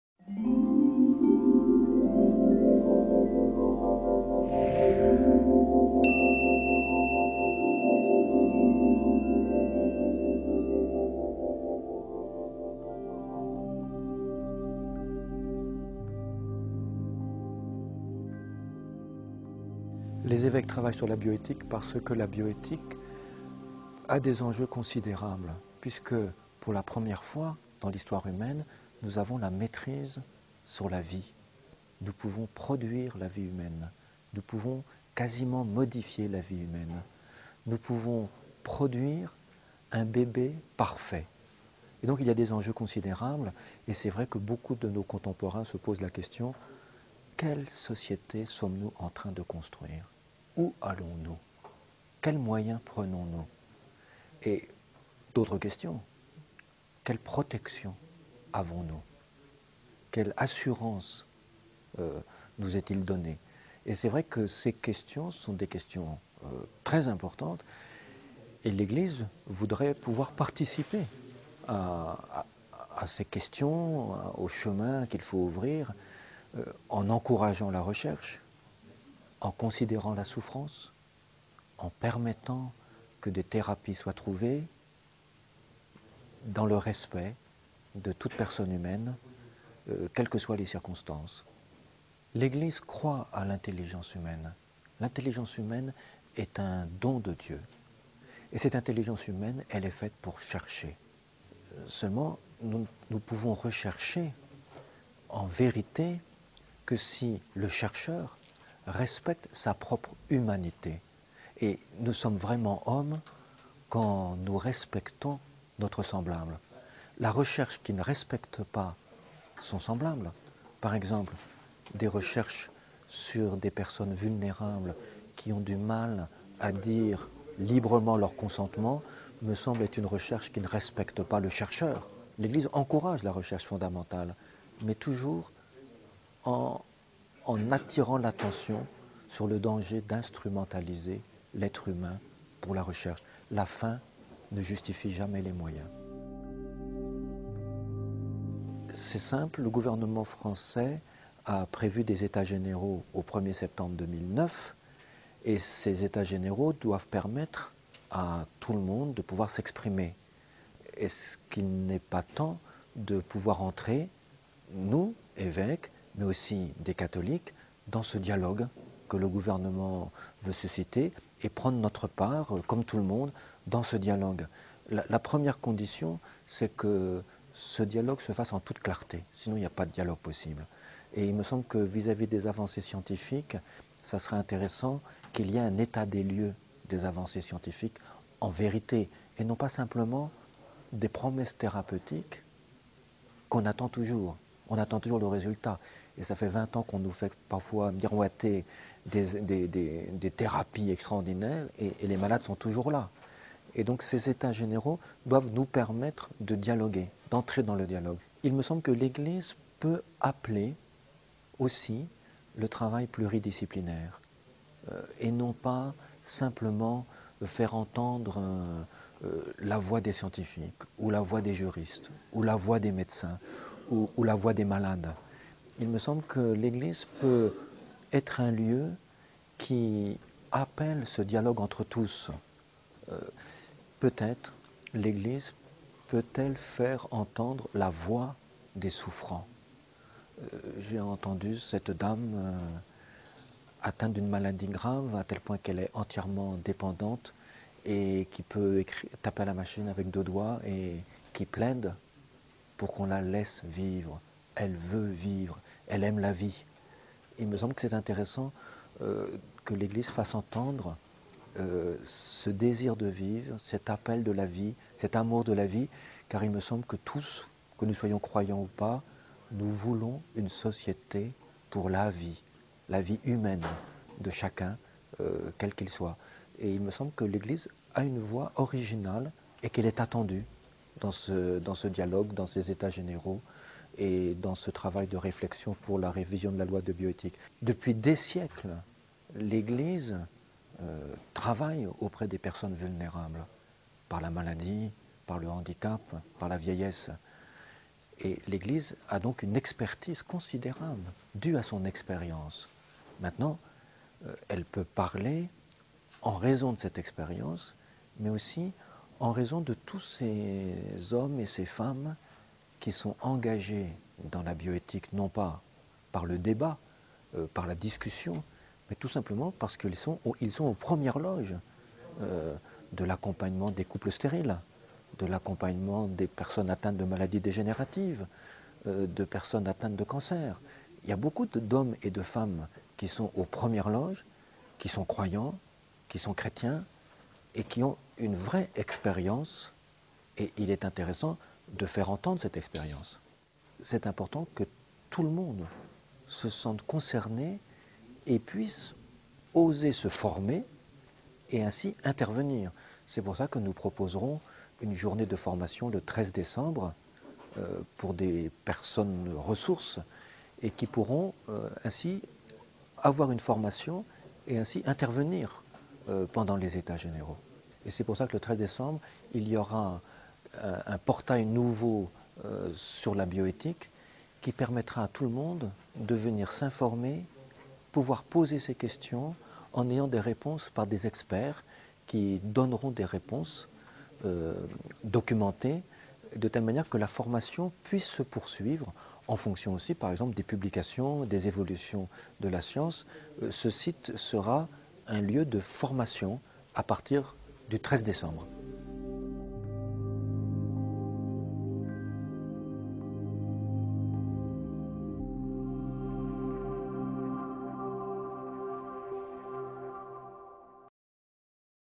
Mgr Pierre d'Ornellas parle de la bioéthique (Catholique)
Bioéthique :
Interview-vidéo de Mgr Pierre dOrnellas, archevêque de Rennes, président du groupe de travail sur la bioéthique.

A la veille des Etats généraux de la bioéthique prévus en 2009, qui ouvriront des débats en vue de la révision de la loi relative à la bioéthique, Mgr Pierre d'Ornellas revient sur l'enjeu de cette révision et sur la contribution de l'Eglise à cette réflexion et au dialogue sur un domaine qui touche la dignité de la personne humaine.